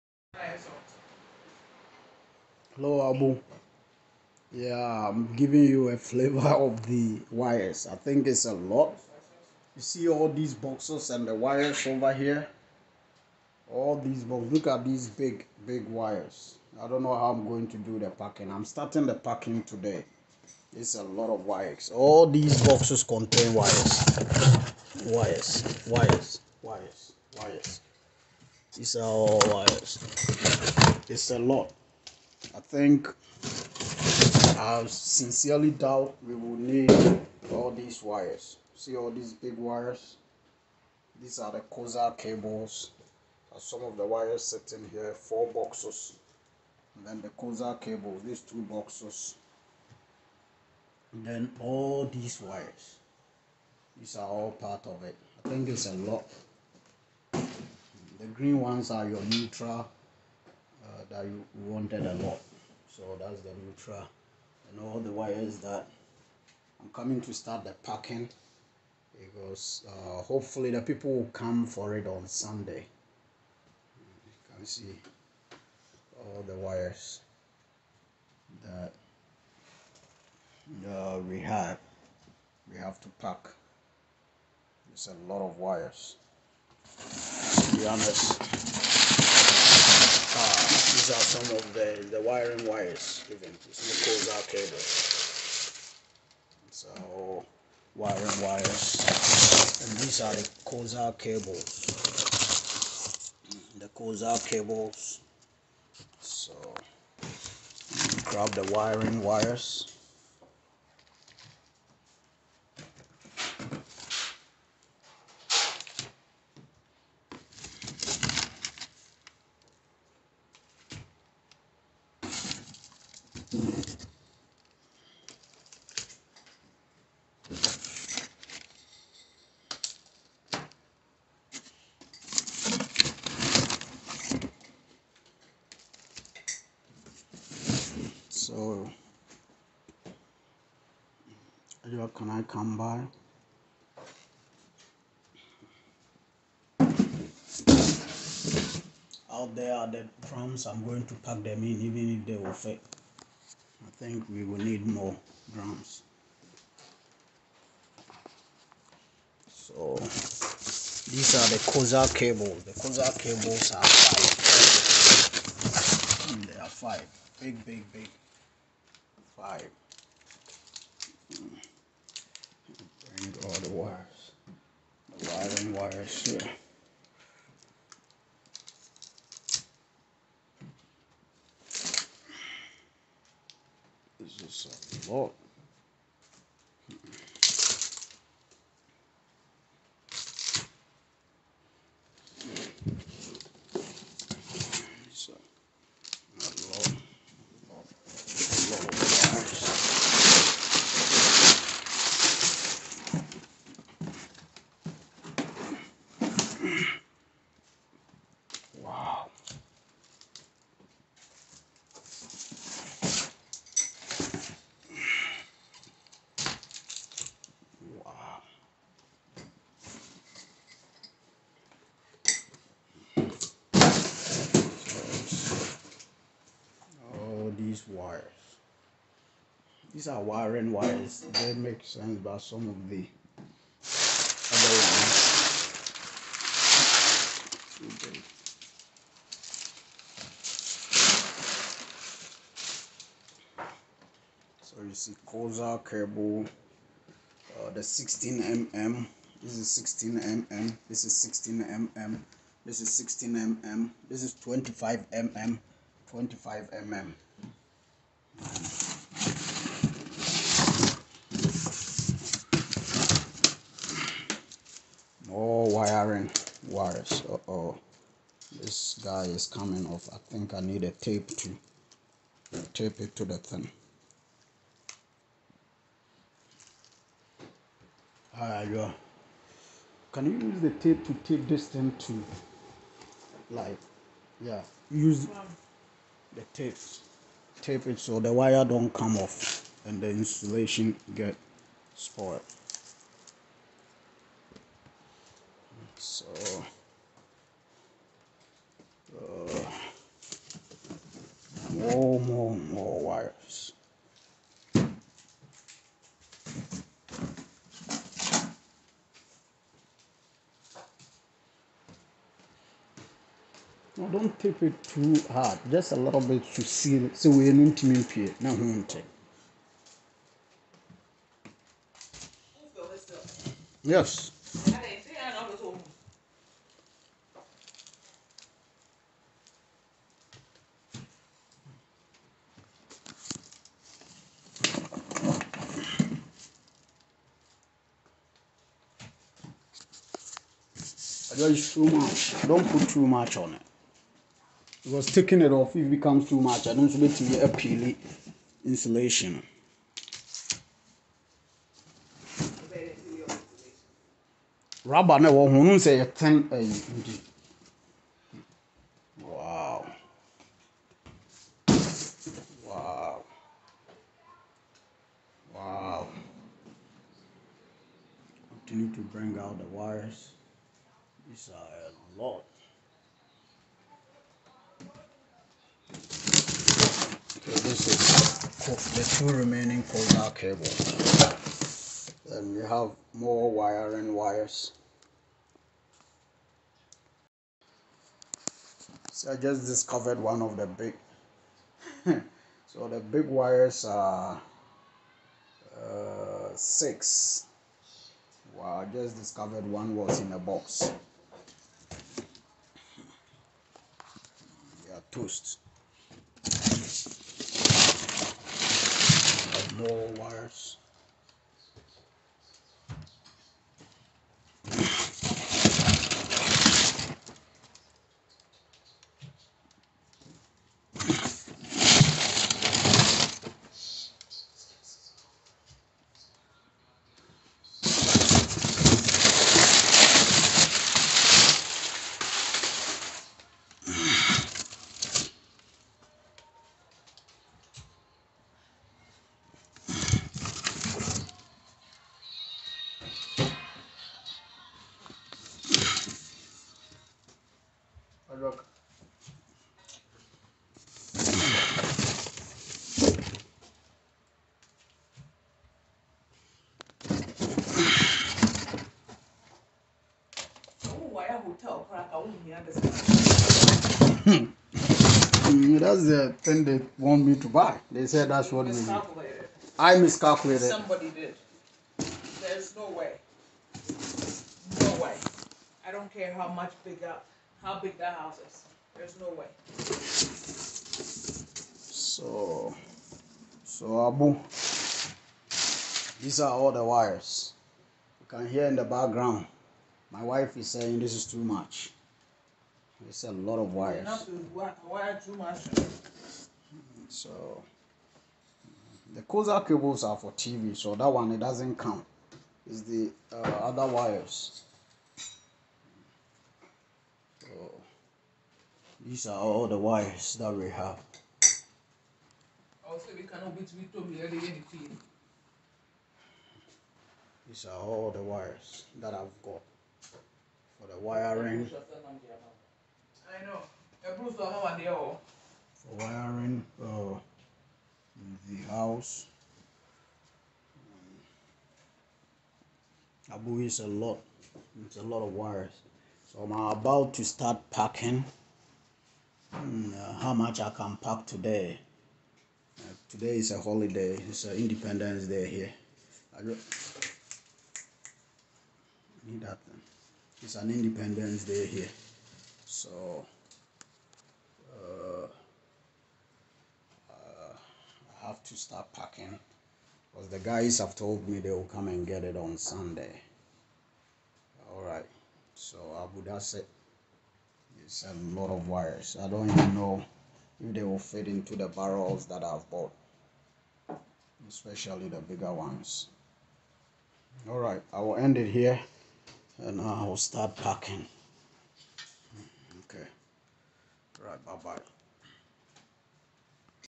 Wires for Wiring in Berekuso, Ghana 🇬🇭 - Final Packing for Shipment
Wiring Wires For Berekuso Ghana 🇬🇭. The Final Packing for Shipment to Ghana